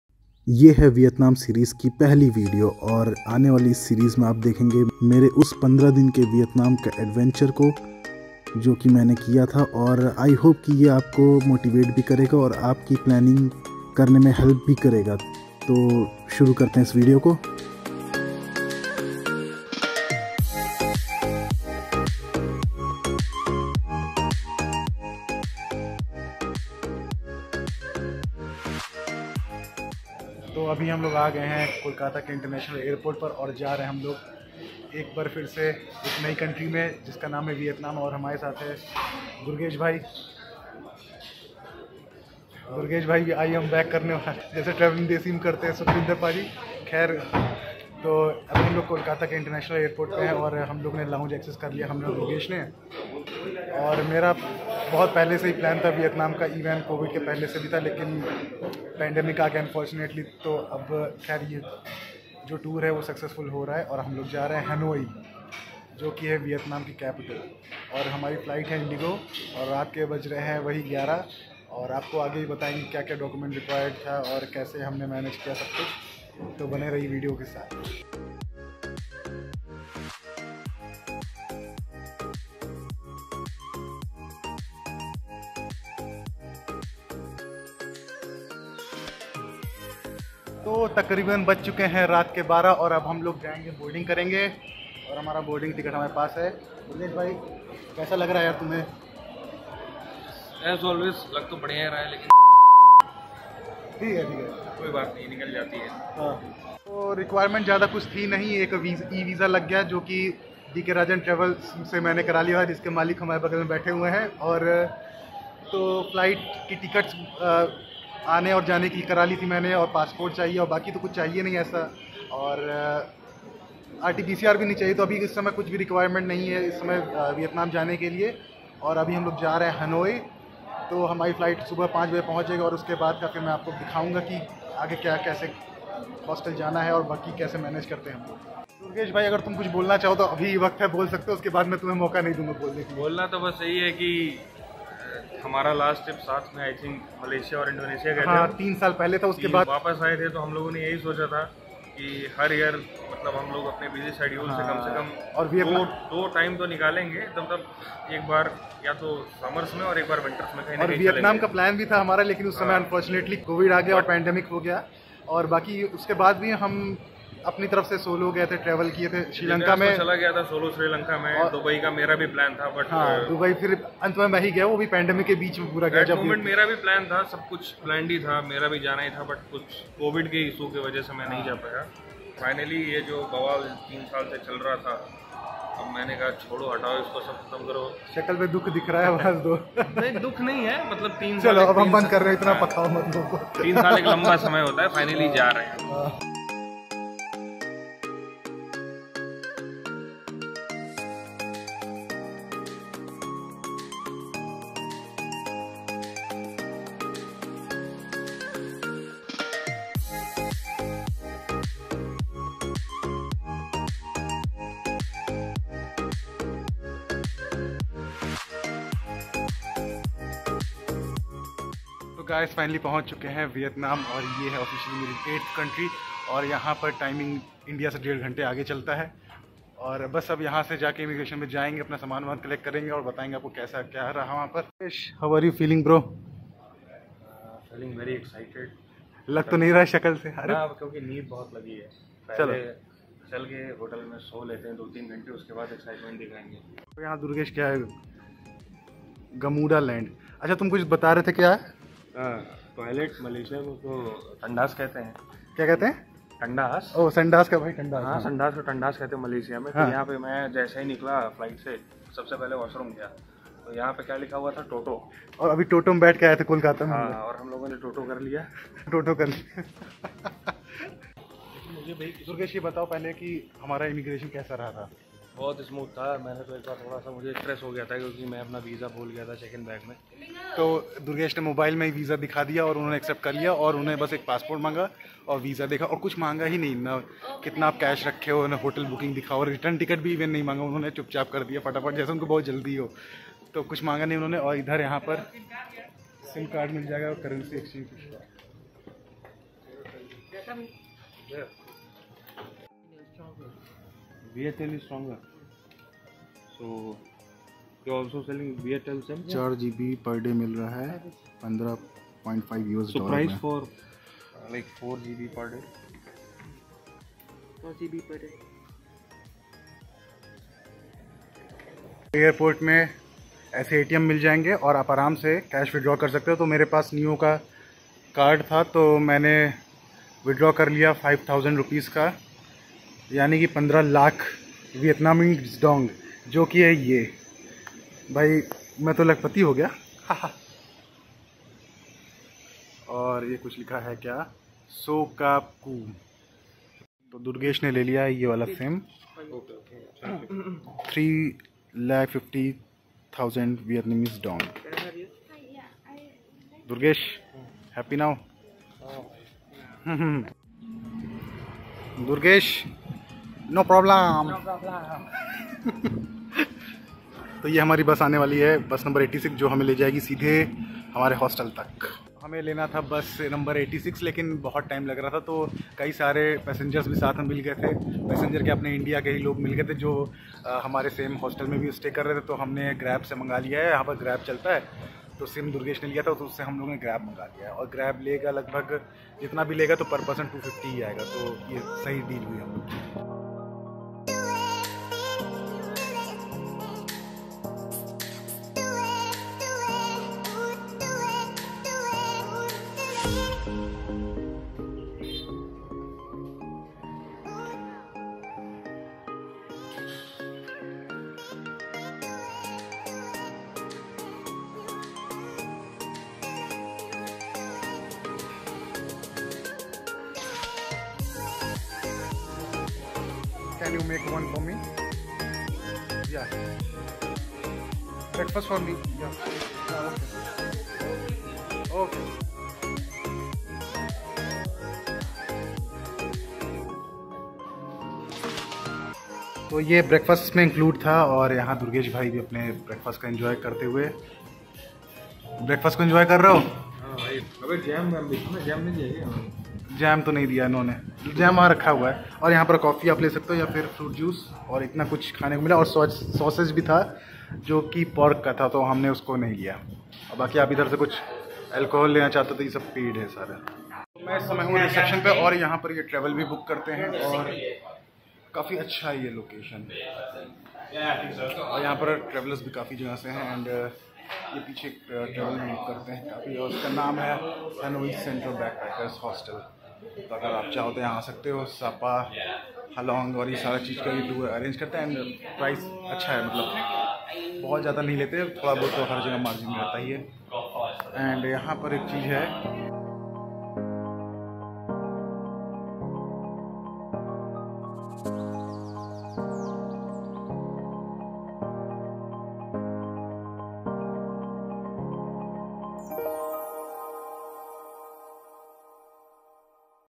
India to Vietnam🇻🇳| Required documents? | Best Party hostel in Hanoi| Free Beer🍺
Hello everyone
 I went to Vietnam with my childhood friend Durgesh, Watch the video it will help you for ur travel..
DK RAJAN TRAVELS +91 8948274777

What do I use :
Primary Camera
Memory Card 128 GB
HDD 2 TB